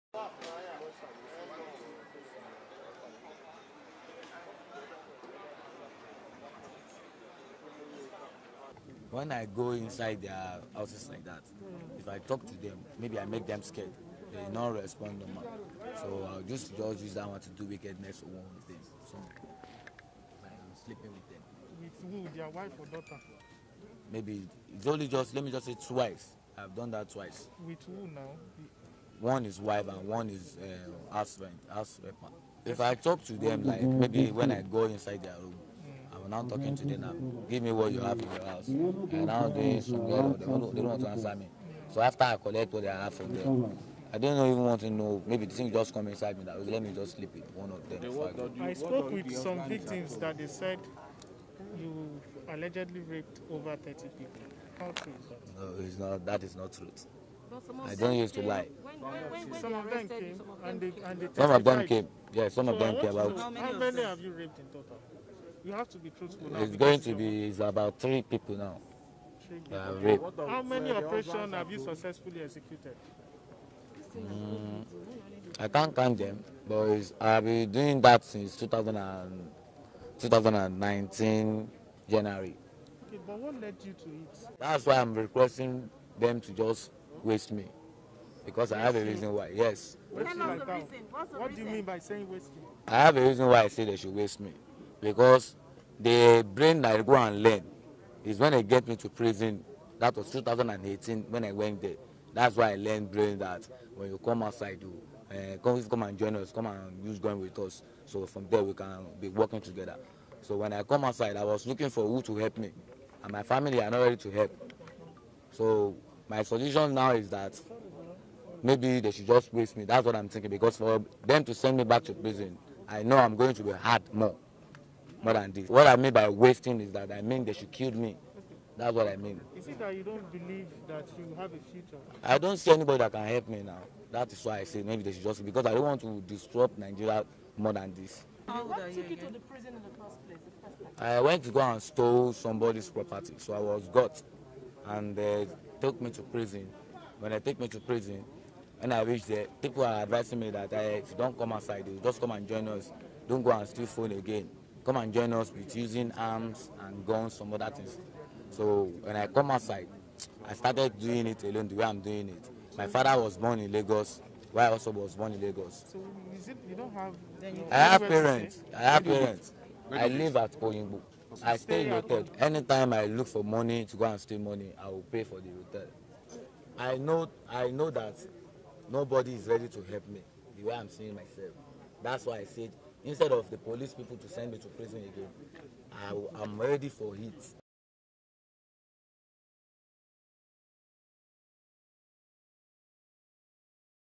An Armed Robber And A Rapist Begs Police Officer To Kill him
An armed robber and a rapist, Adeniyi Ajayi wants police officers, to end his life because he doesn't want to cause more harm and havoc in Nigeria.